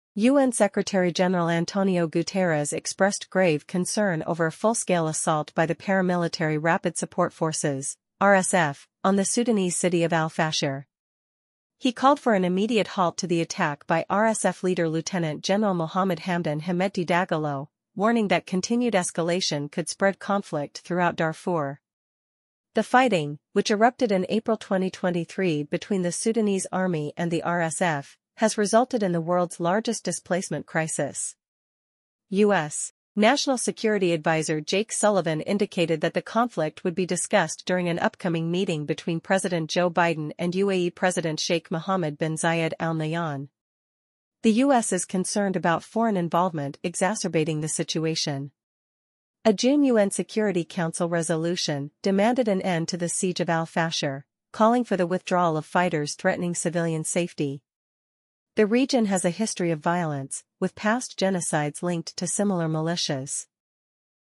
Guterres Condemns RSF Assault on Al-Fashir as Conflict Escalates in Sudan
UN Secretary-General António Guterres has expressed grave alarm over reports of a full-scale assault by the paramilitary Rapid Support Forces (RSF) on the Sudanese city of Al-Fashir. He called for an immediate halt to the attack, warning of escalating violence and its dire consequences for civilians in the Darfur region.